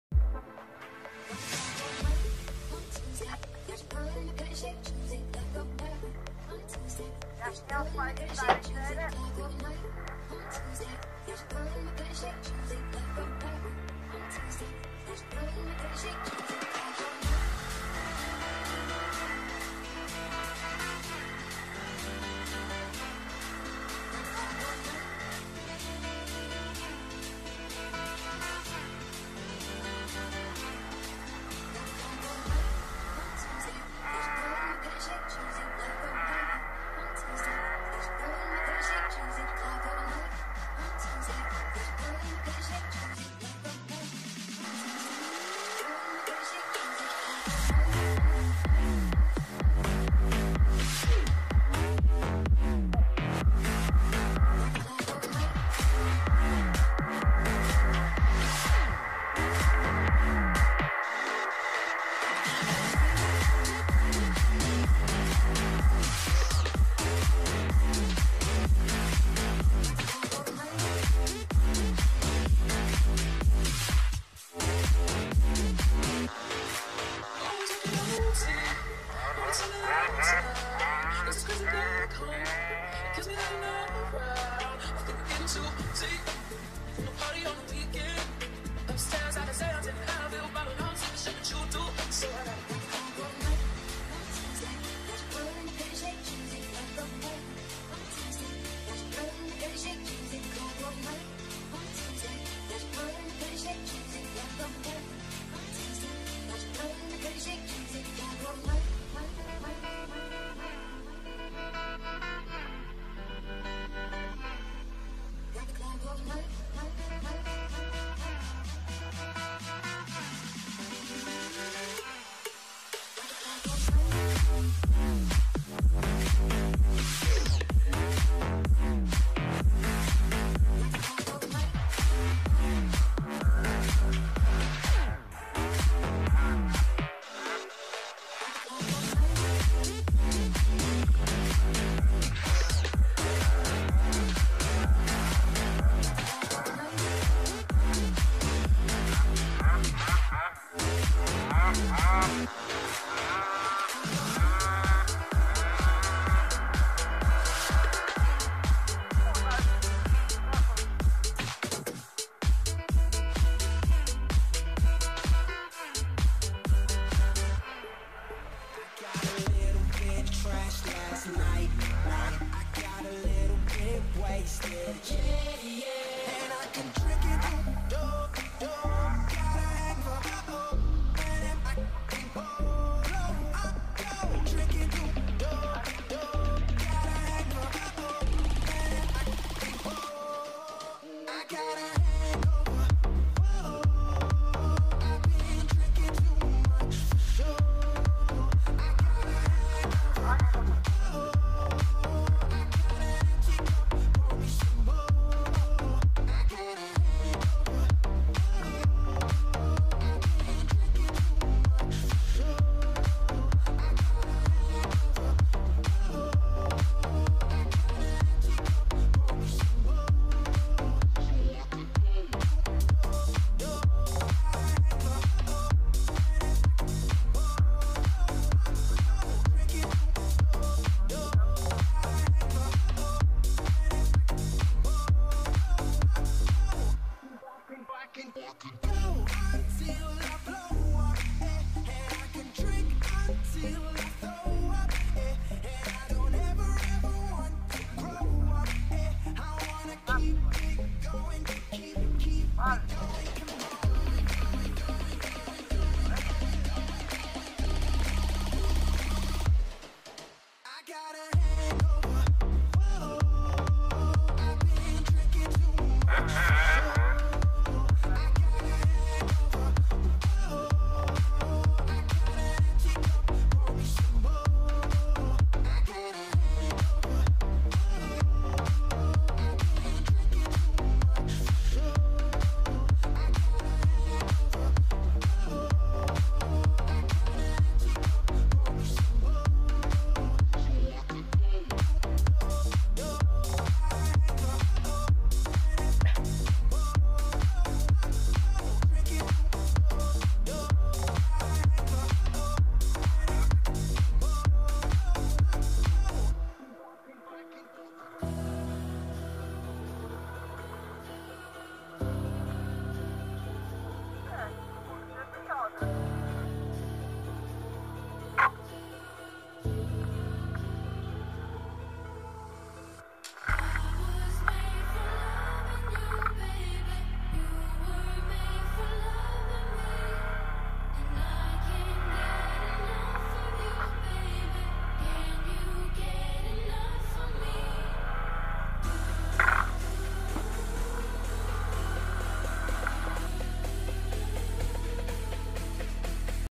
Honda Szybsza niż wygląda !!! Jutro Motovlog.
Nagranie z kolegi kamerki który jechał na hondzie crf110 a tóż przed nim ja na Ktm sx85